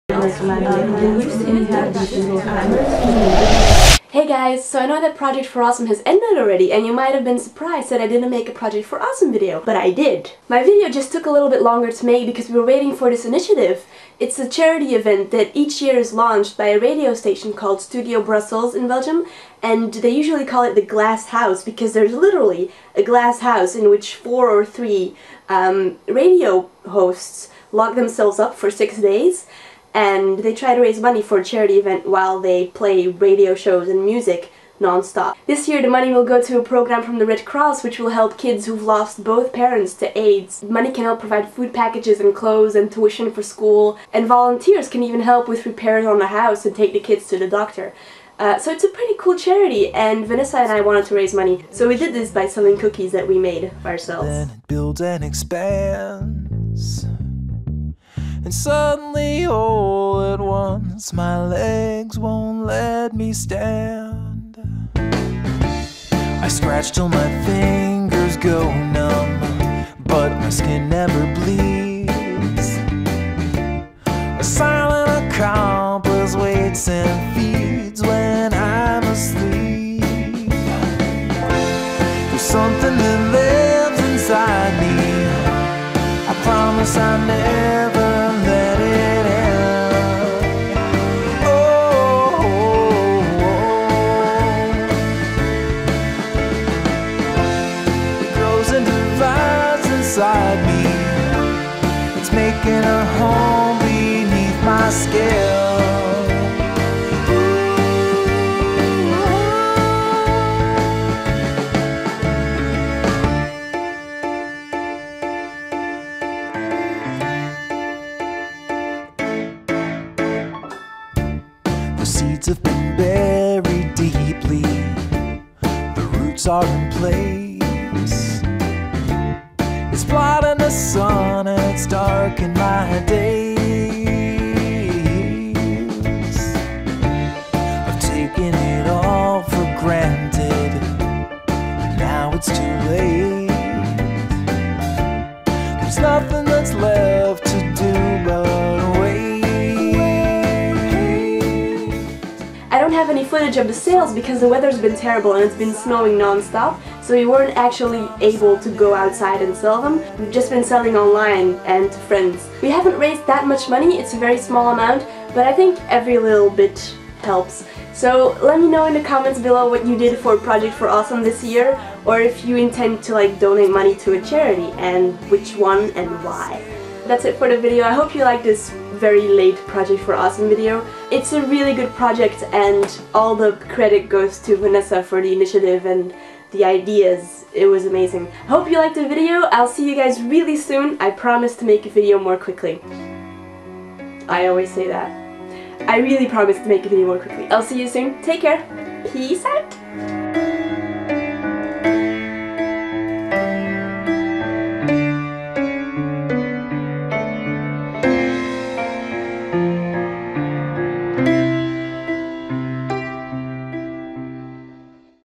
P4A: Cookies For Life
Our little fundraiser: cookies for life!

Apologies for crappy editing, I had to edit on my little netbook :D